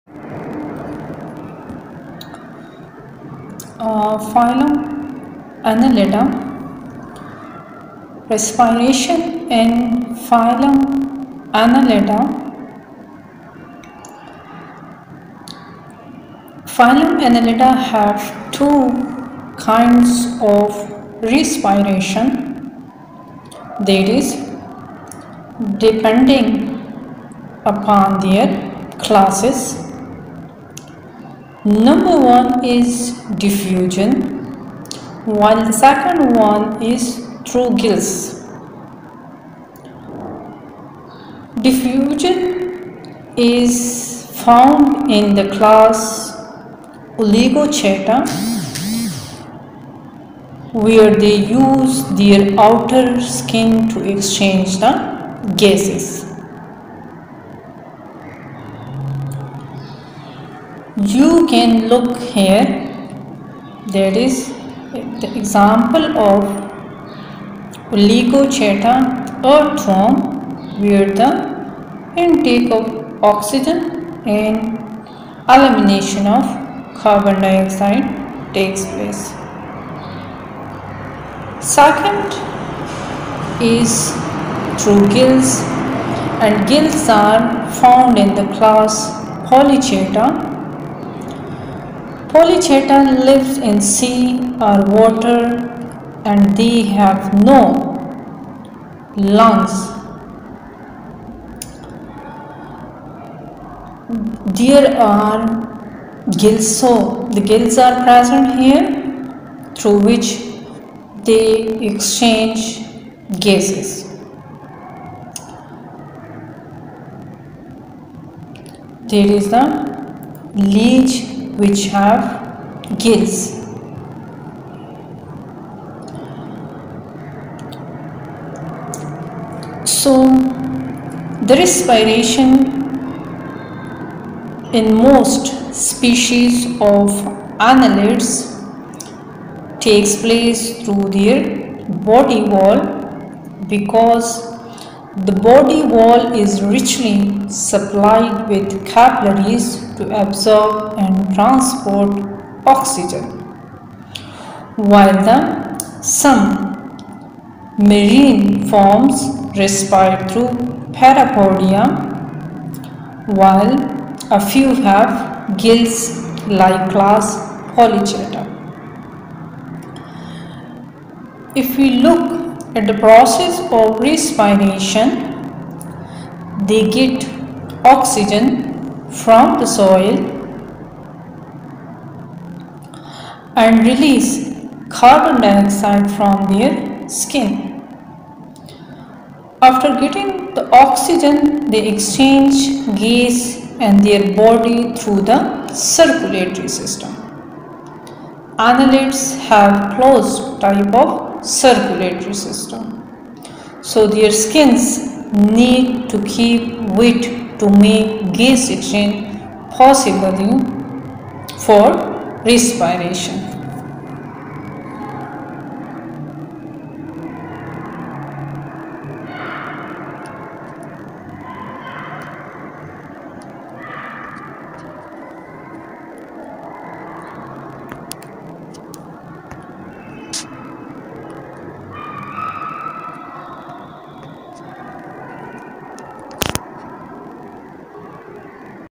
PHYLUM ANNELIDA: Respiration and its types
Gas exchange generally takes place through the skin, but it may occur through gill filaments in some polychaetes or through the rectum of aquatic oligochaetes. Although oxygen may be transported directly in the blood, it is usually carried by a respiratory pigment, either hemoglobin or chlorocruorin.